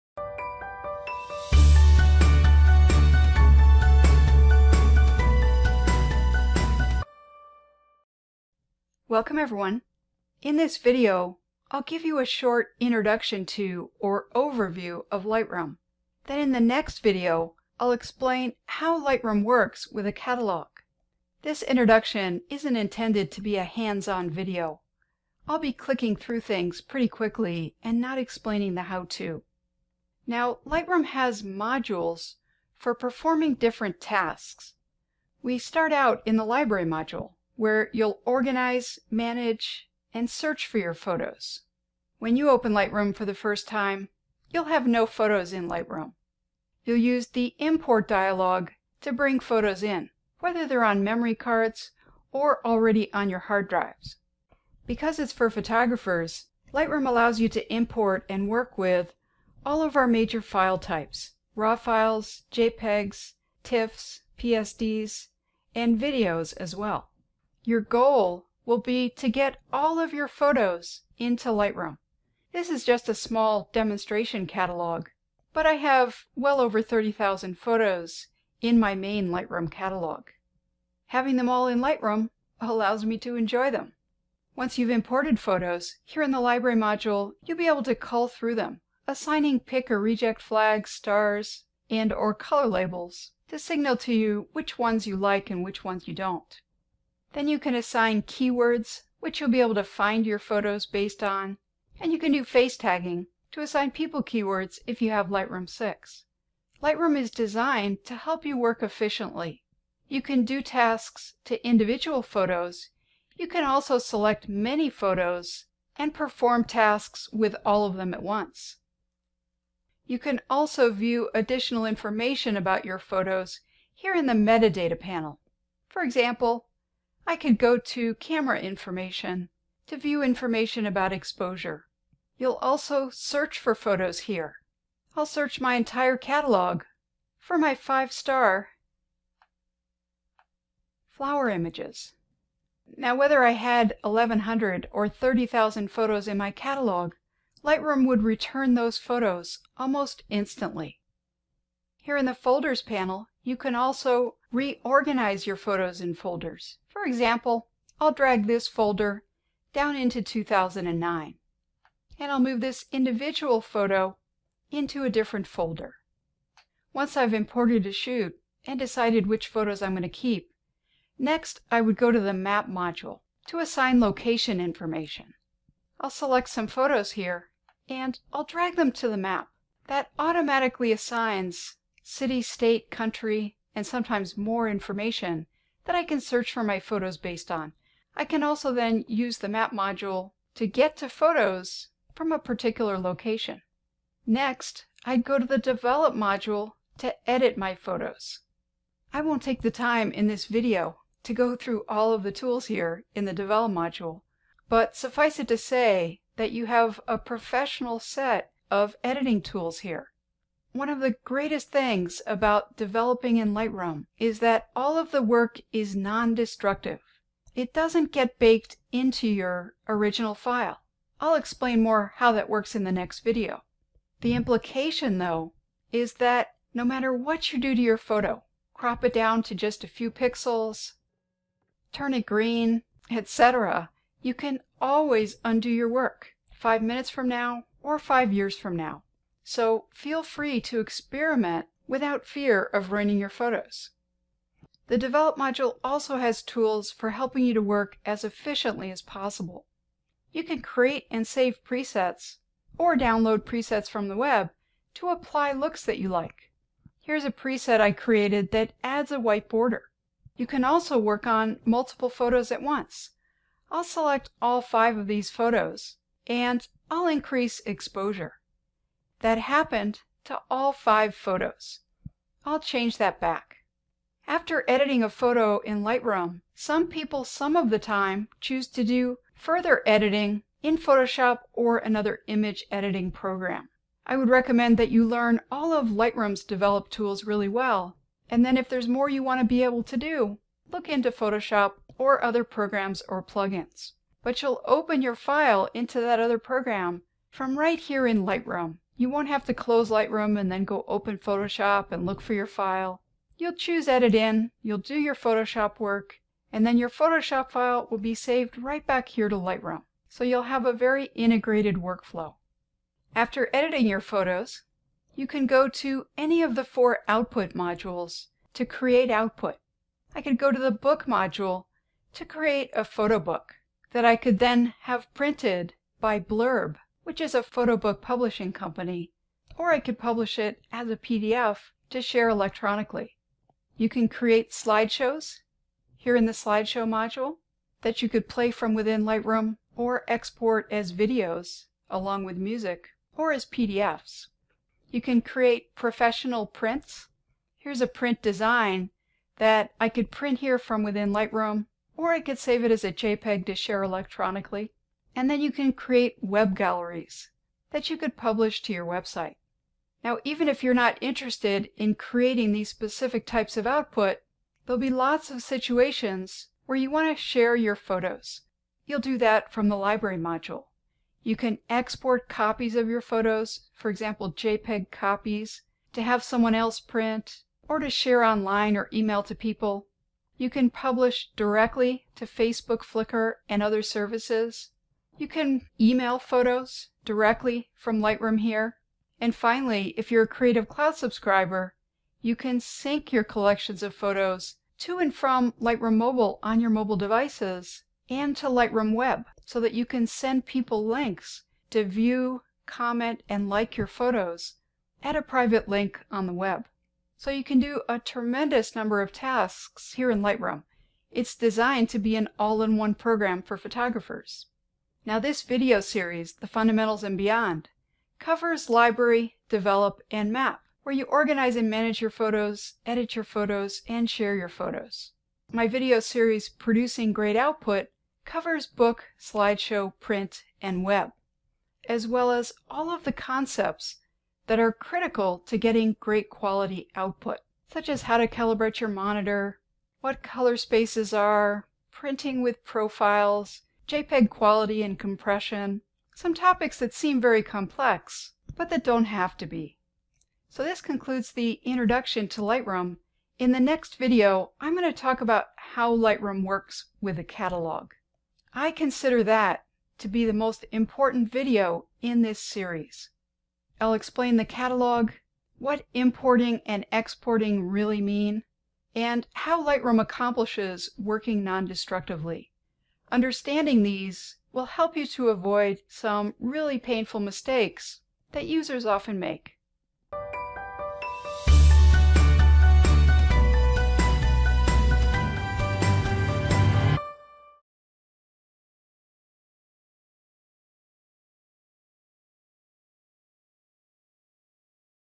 New to Lightroom? Watch This Brief Introduction to Lightroom
For beginners, Laura Shoe presents a brief overview of Lightroom - learn what it is and how it is organized in this second video in her widely-acclaimed 15+ hour Lightroom Fundamentals & Beyond video series